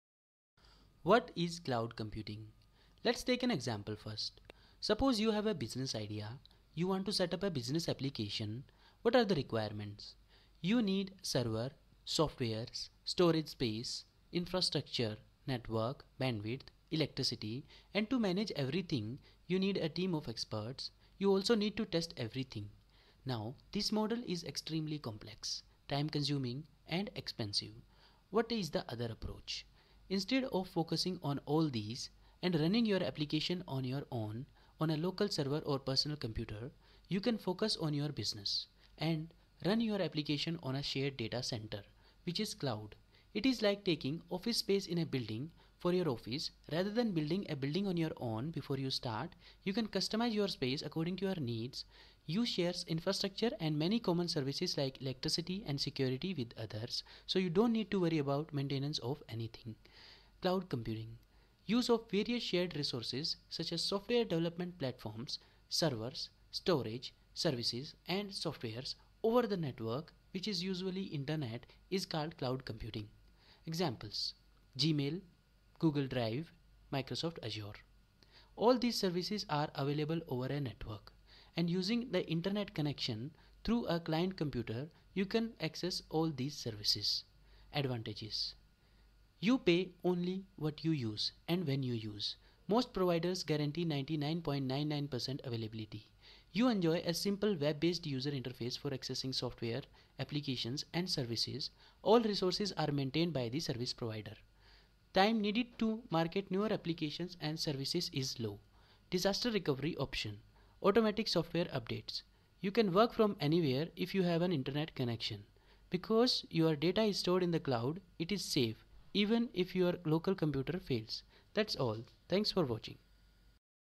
What is Cloud Computing (Computer Science)
Description of cloud computing and how it works